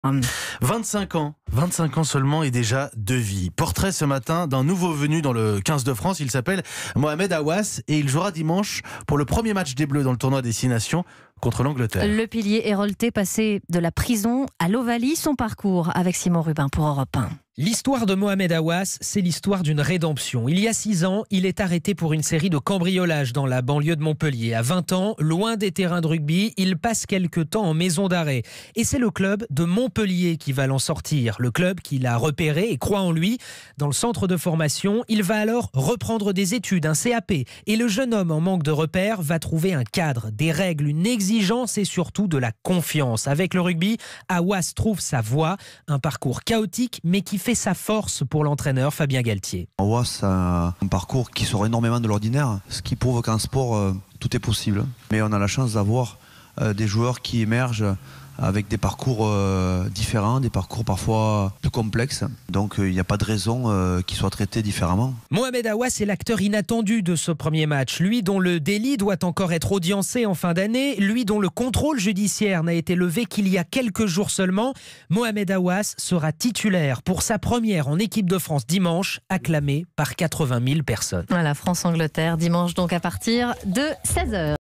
Mohamed Haouas, de la prison au XV de France
Passé de la prison au XV de France, Mohamed Haouas est le nouveau venu chez les Bleus. Il joue au rugby à Montpellier et a été nommé titulaire par Fabien Galthié dans l'équipe, qui ouvrira le Tournoi des Six Nations dimanche au Stade de France. Un parcours atypique dans le monde du sport de haut niveau